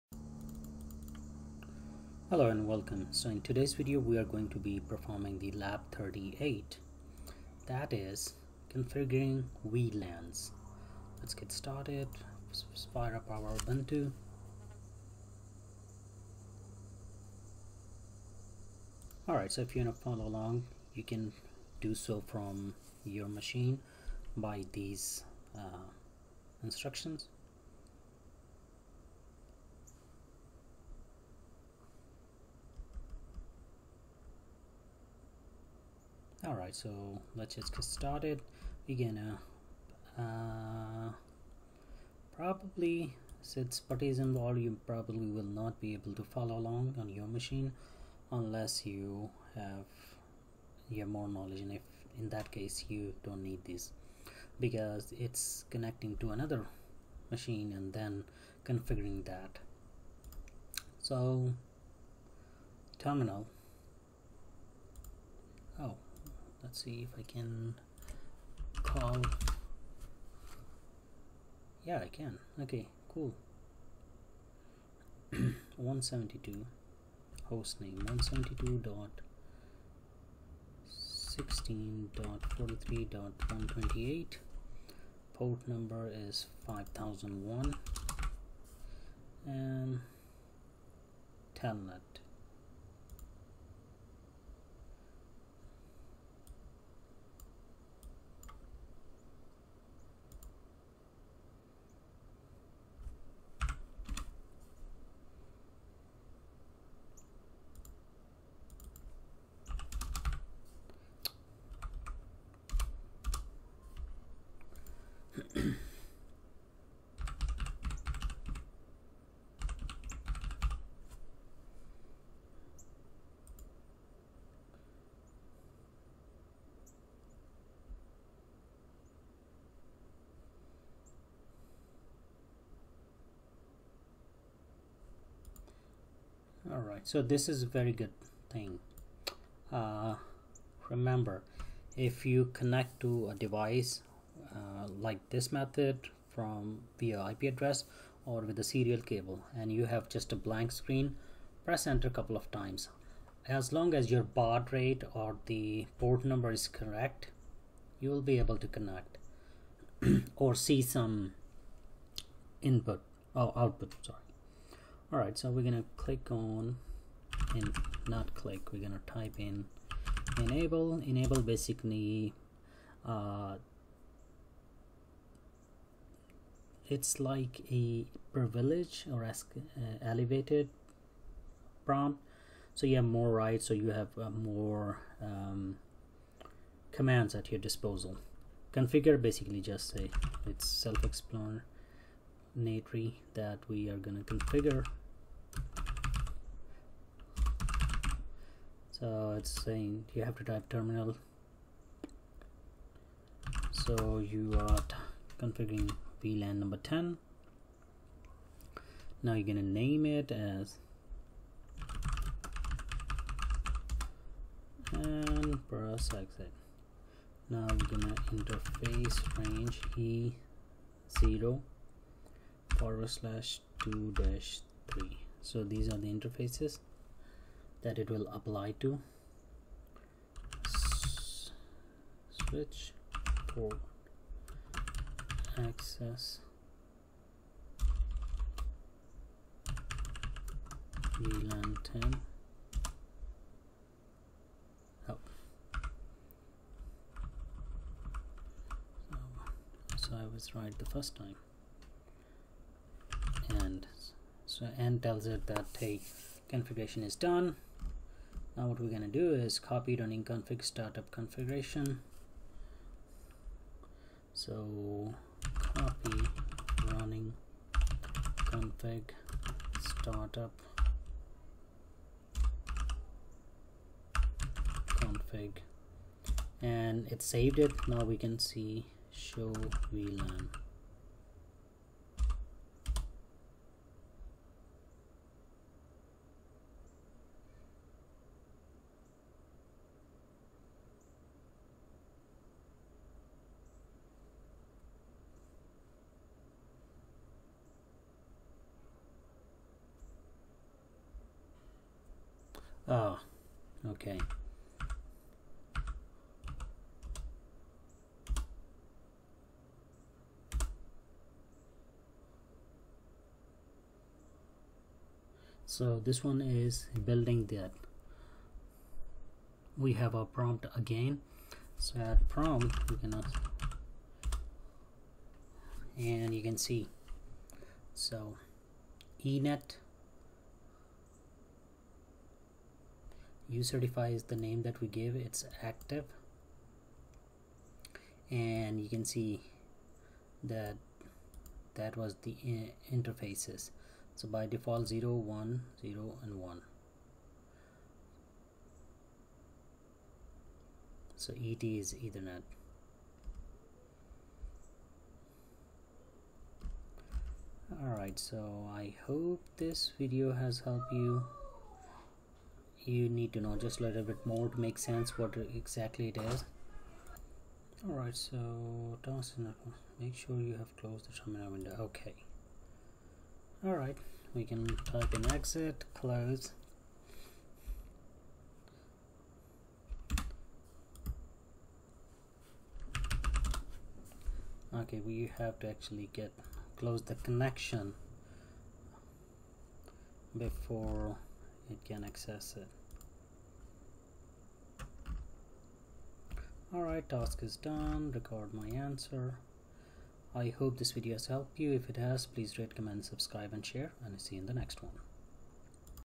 Configuring VLANs
In this lab, you will learn to configure virtual local area networks (VLANs). VLANs are logical subdivisions of a switch that segregate ports from one another as if they were in different LANs. VLANs offer another way to add a layer of separation between sensitive devices and the rest of the network.
To configure VLANs